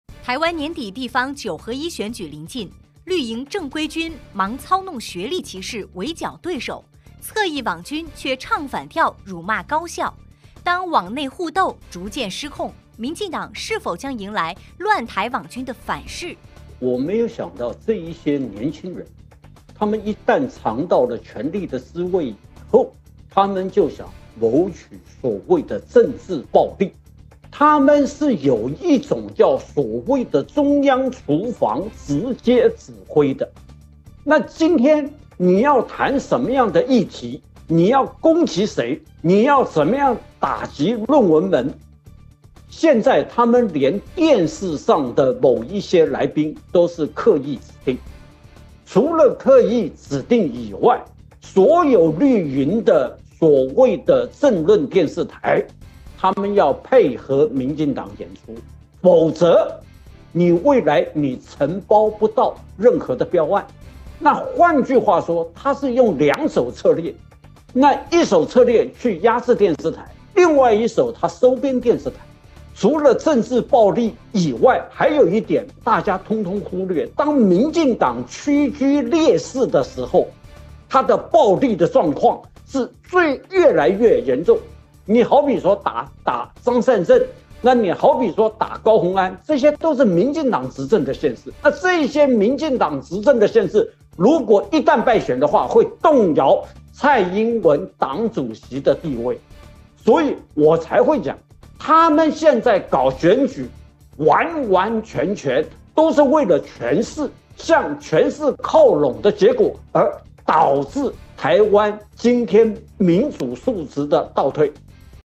【兩岸直航】張友驊：民進黨利用網絡霸凌謀取政治暴利
台灣年底地方“九合一”選舉臨近，綠營“正規軍”忙操弄學歷歧視圍剿對手，“側翼網軍”卻唱反調辱罵高校，當“網內互鬥”逐漸失控，民進黨是否將迎來“亂台網軍”的反噬？台灣時事評論員張友驊表示，民進黨通過利用網軍、操控電視台等手段謀取“政治暴利”，為了權勢而選舉，導致台灣地區民主素質倒退。